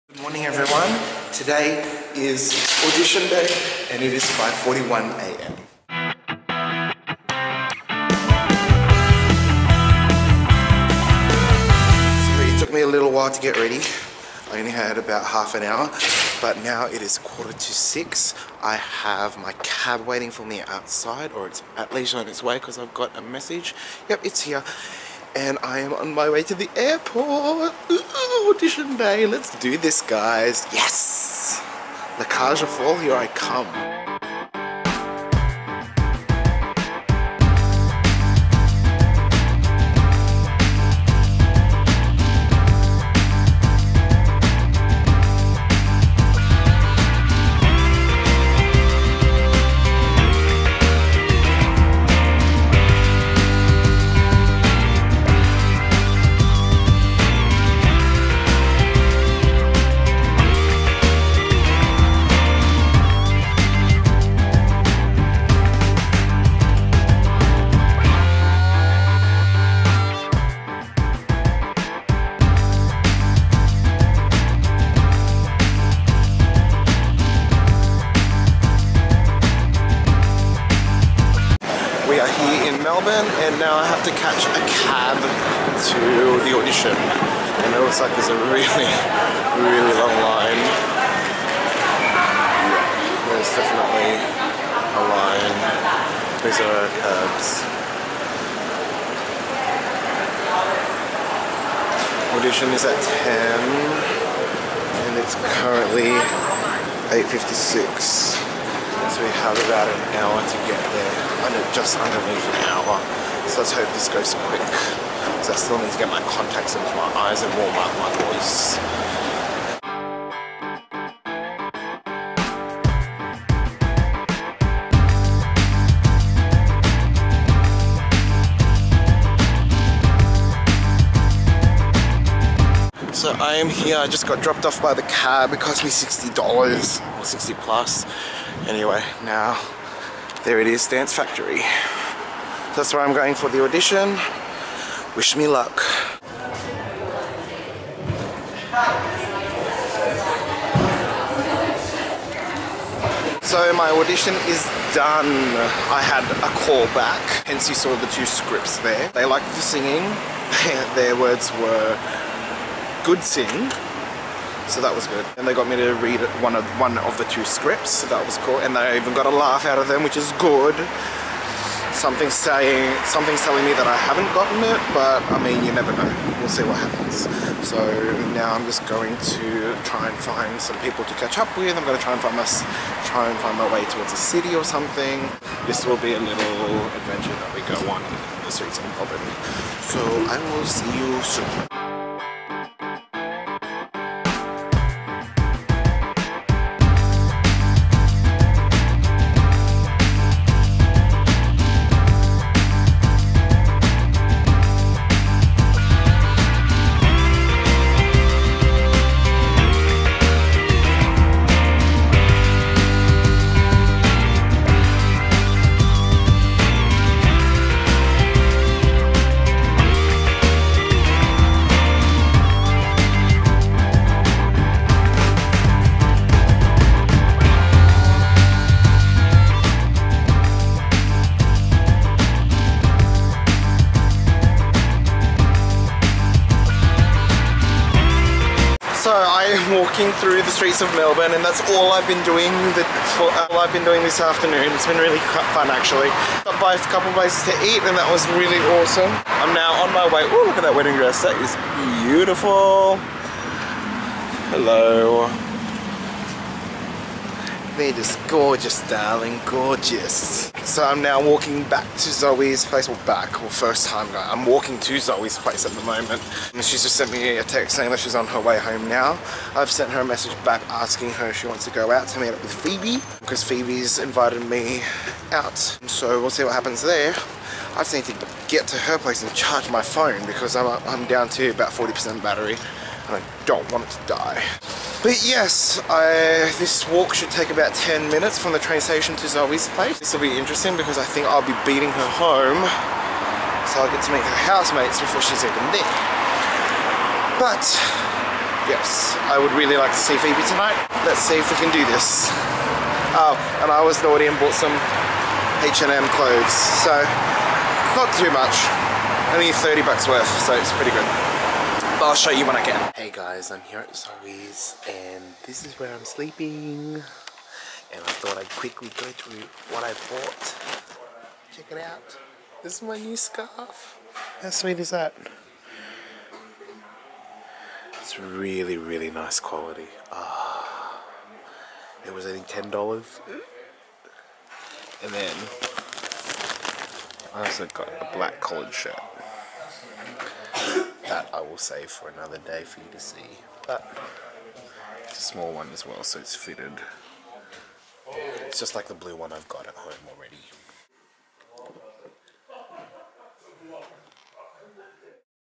Audition In Melbourne
Today we travel to Melbourne to audition for The Production Company's last show of the 2014 season, La Cage Aux Folles.